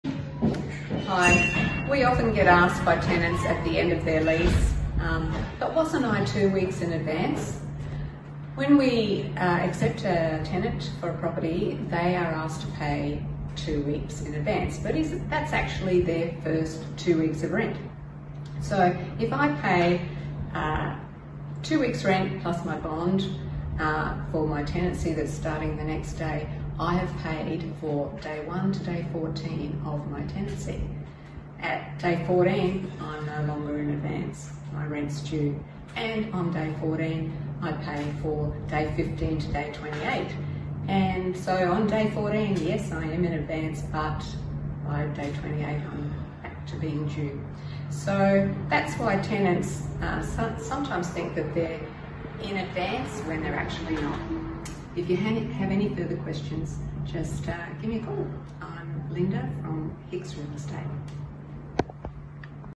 Understanding two weeks' rent advance for tenants
➡️ When we accept tenant for a property, they are asked to pay two weeks' rent in advance.
➡️ The initial payment covers the first two weeks of rent, not an additional two weeks.

📞Anything unclear, don't hesitate to give us a call - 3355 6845